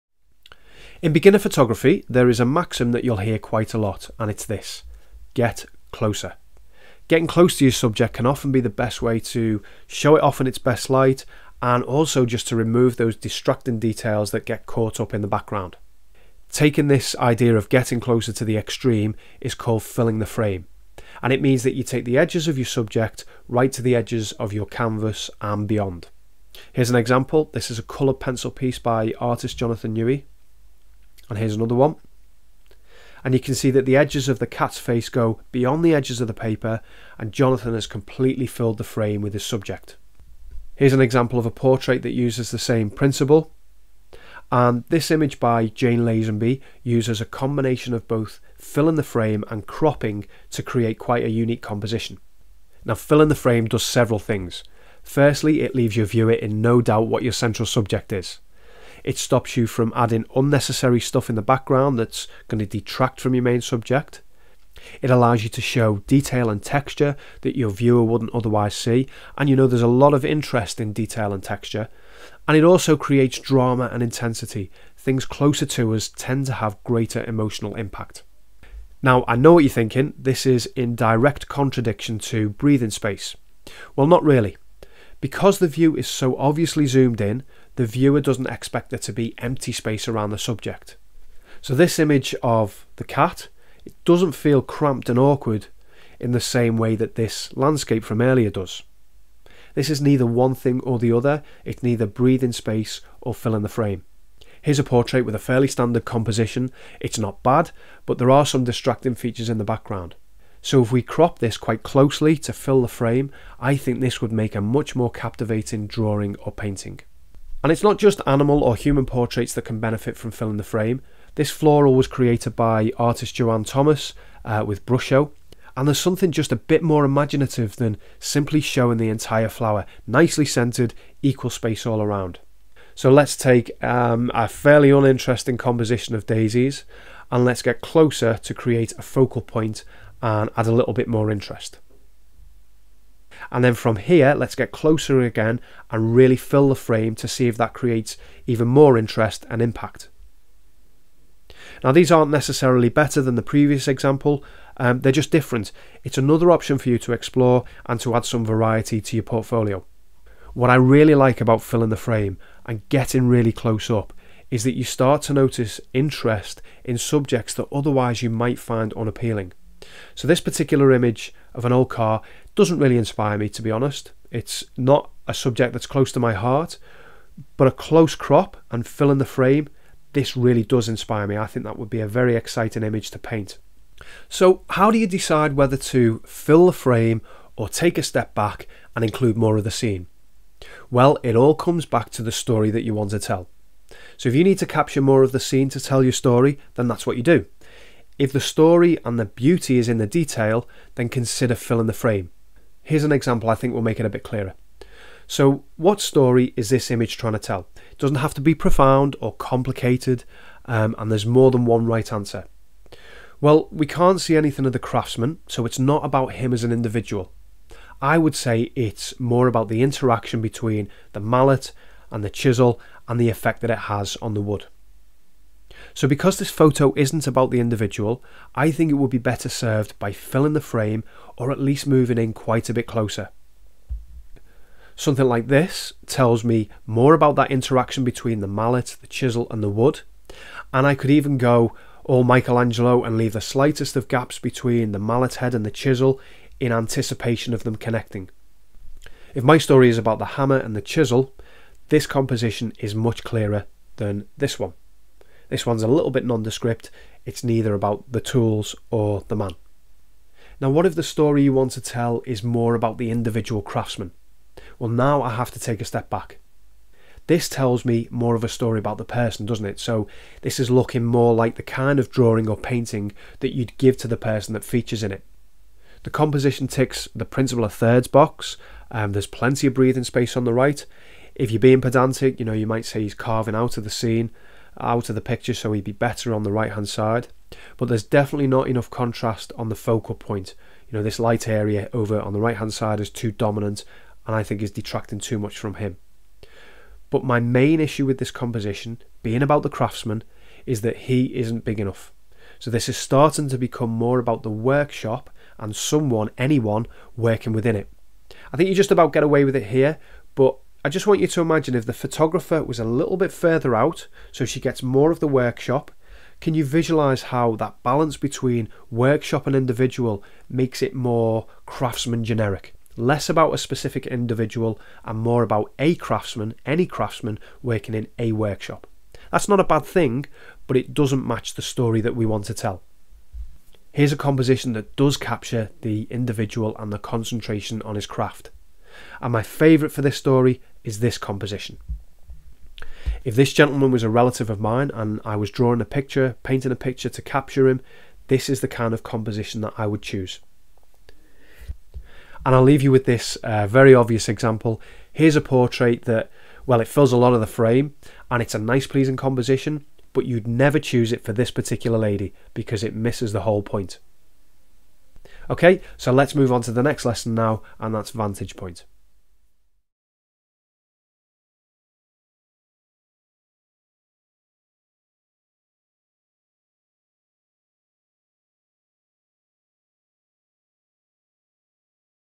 Composition tip for your next drawing or painting
Using the compositional technique of filling the frame can create a dramatic, engaging piece of art. This video shows when, and when not to use it, with lots of examples from art and photography.